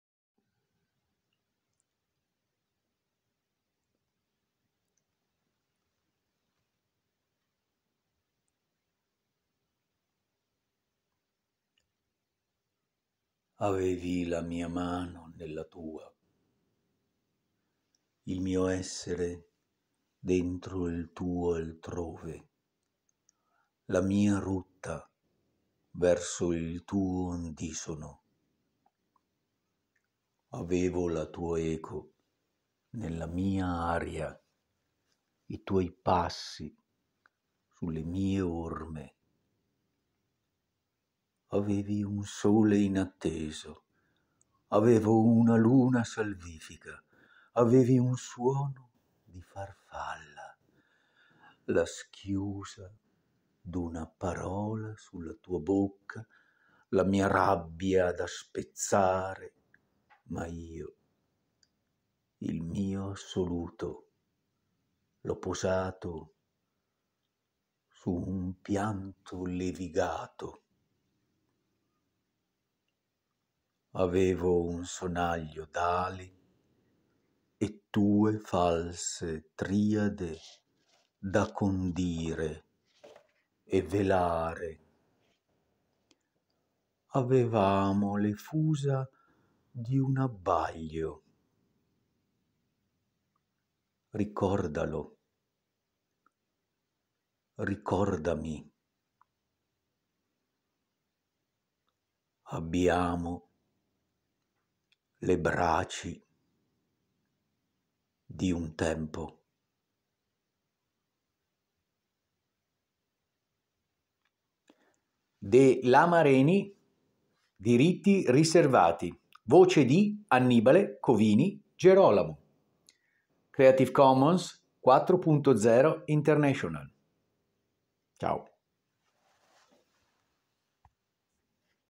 Avevi la mia mano, poesia de: La Mareni; voce di Annibale Covini Gerolamo 🎭❤️🌻
poesia de La Mareni: Avevi la mia mano; voce di Annibale Covini Gerolamo 🌼🍀🎭